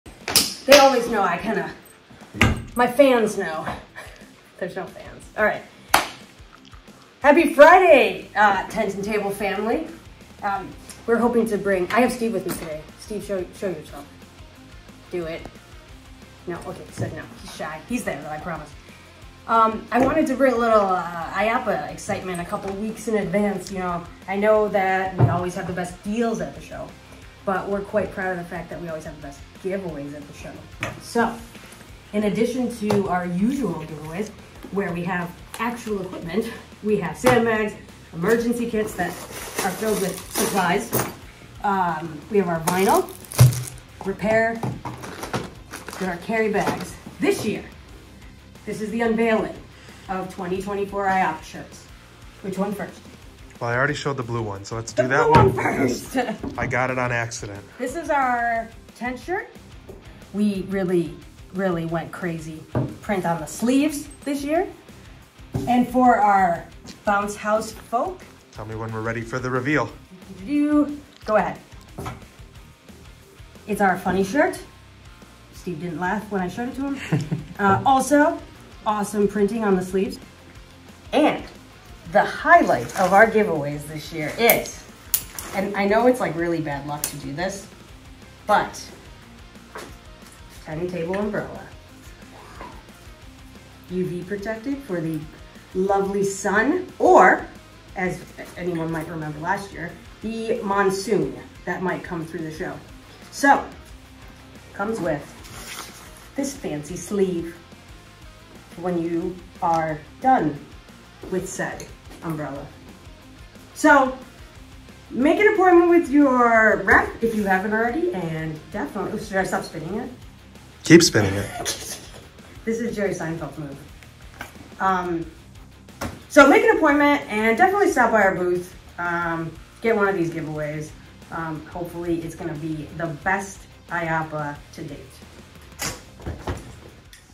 IAAPA 2024 Giveaways!
Tent and Table has always prided ourselves on making sure we have cool and exciting new giveaway items for our customers and clients, and this year is no exception! Melissa is here to show you our new tent and bounce house shirts & custom umbrellas that we will be giving away at the IAAPA Expo in a couple weeks. Many more useful and essential items will be given away such as sandbags, emergency kits, vinyl repair kits, and carryall bags.

Make an appointment & stop by our booth at IAAPA 2024 for these great giveaway items!